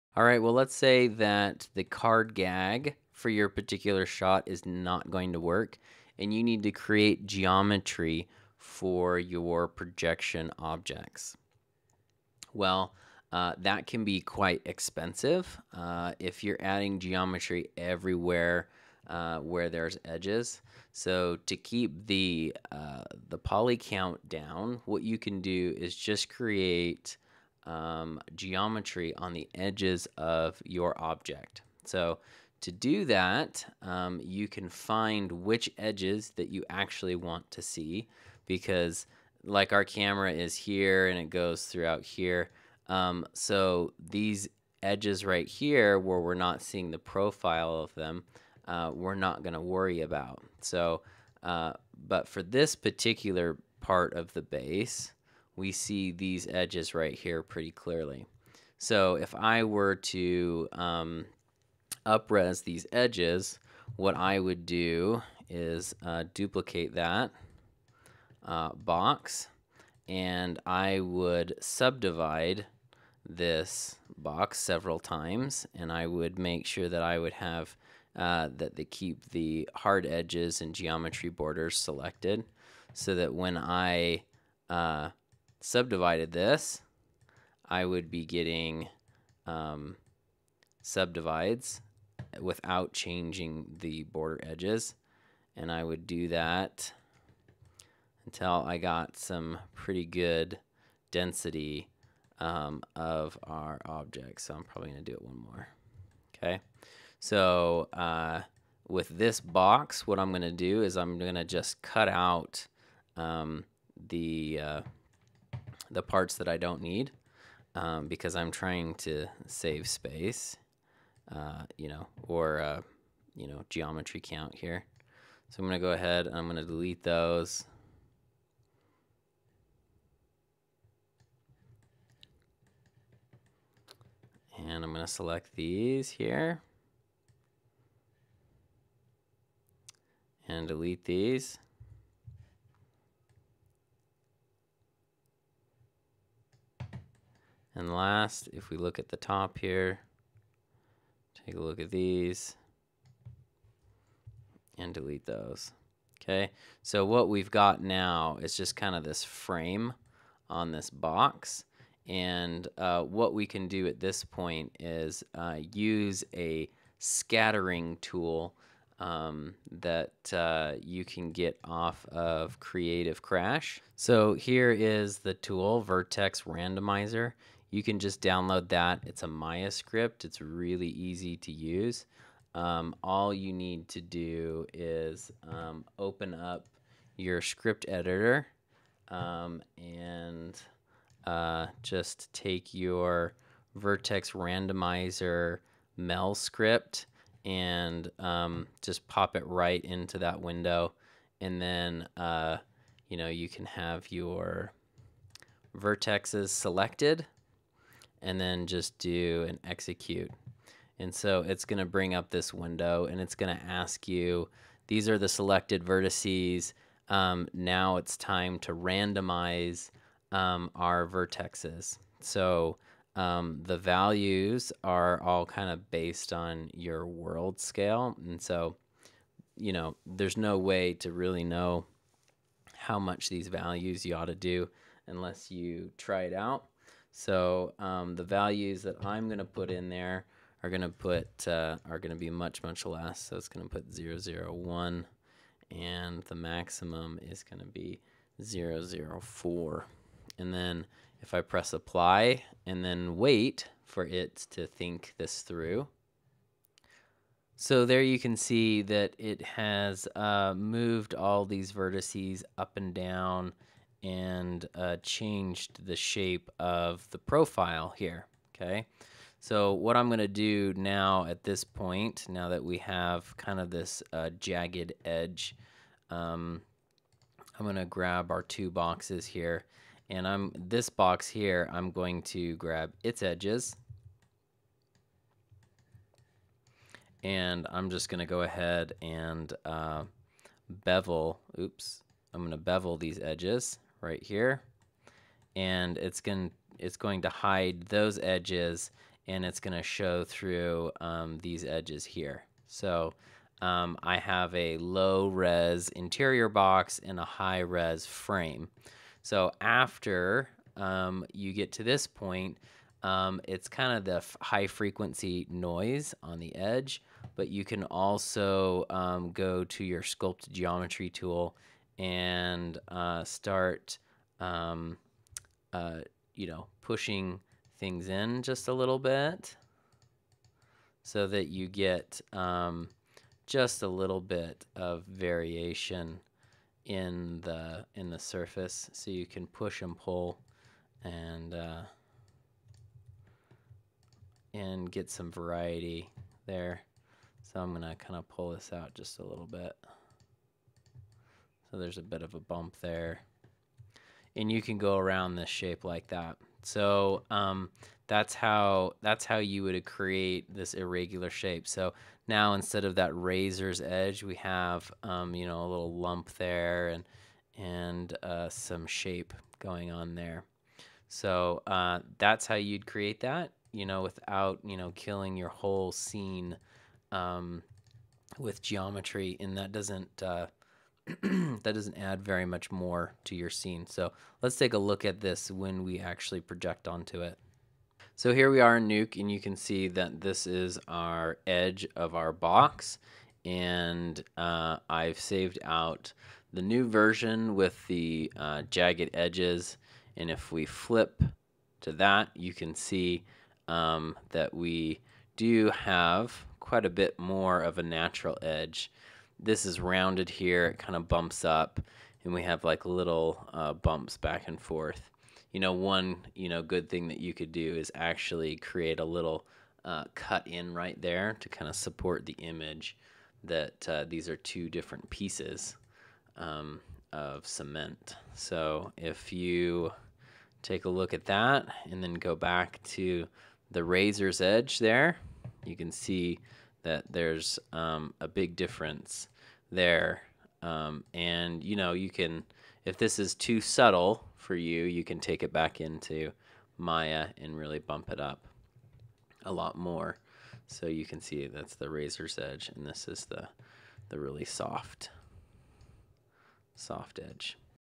Adding Details to Geometry Edges for Projections
This video is part of the “Camera Projection Boot Camp” offered by The Matte Painting for Filmmakers Project (MP4FM)

Sometimes you need to create more detailed geometry to have nice projection edges. Here is a way to create nice edges without adding a crazy amount of geometry.